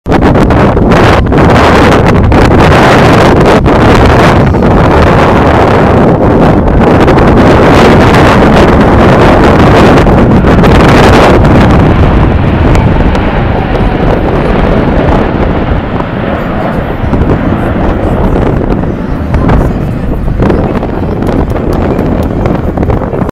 Six planes in triangle formation
Video from the Air Show of the San Francisco Fleet Week 2012.